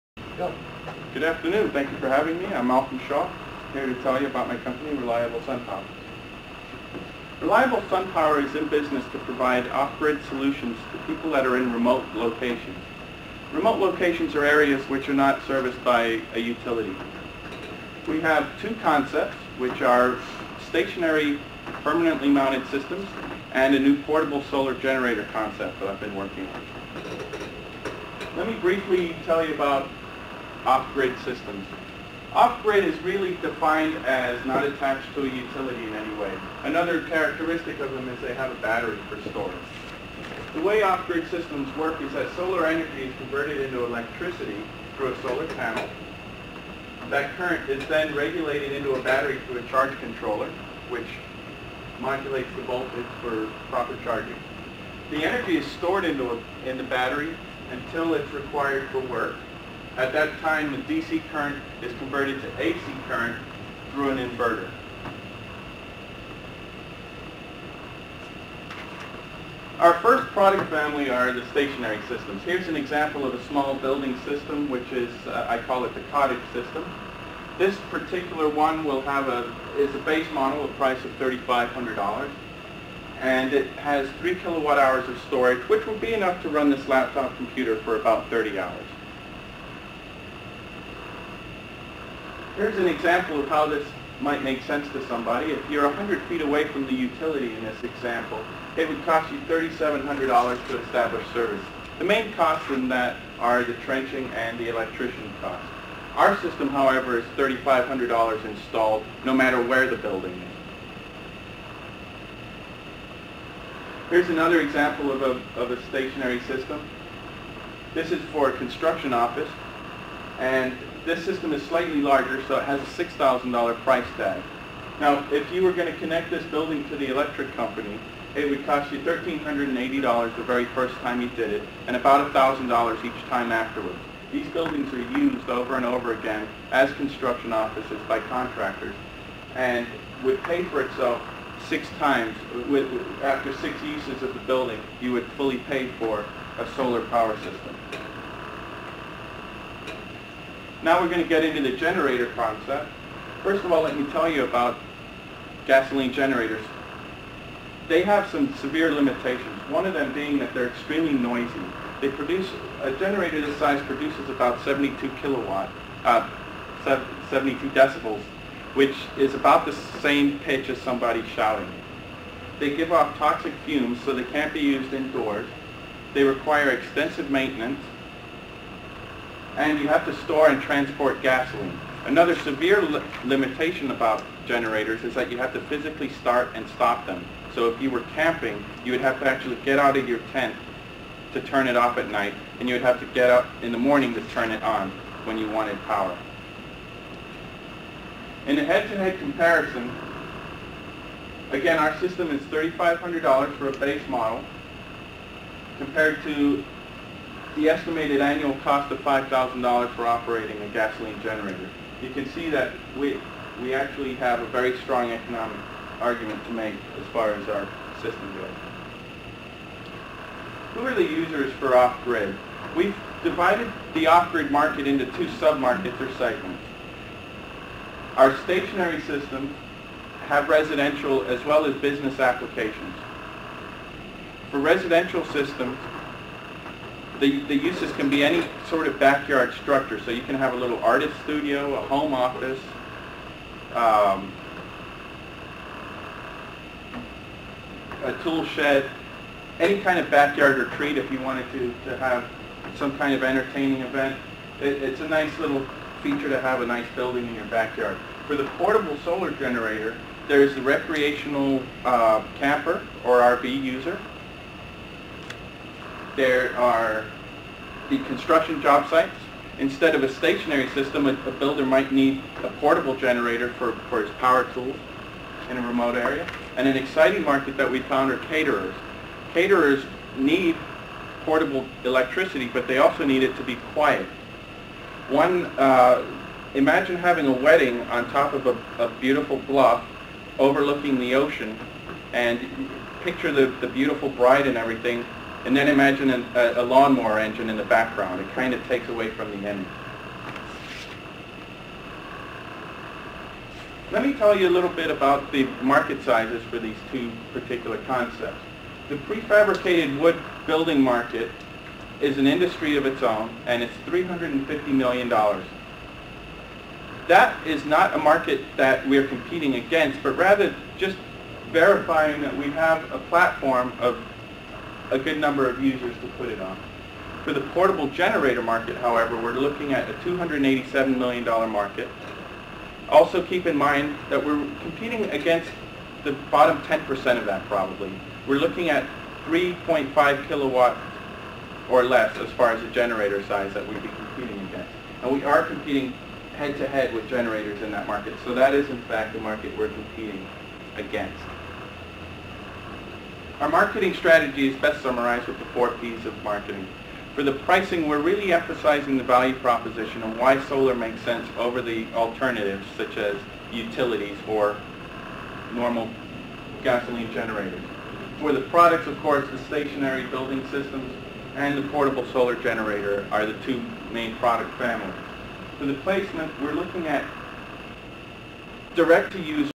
Reliable Sun Power Business Plan Presentation
In 2005 I represented SDSU in a business plan competition in Portland Oregon. This was my practice run. One panel of judges hated it, another panel thought it was the most viable business to get funded. Unlike a lot of the teams, my concept was my own and was not just a group of students presenting a business professor's idea.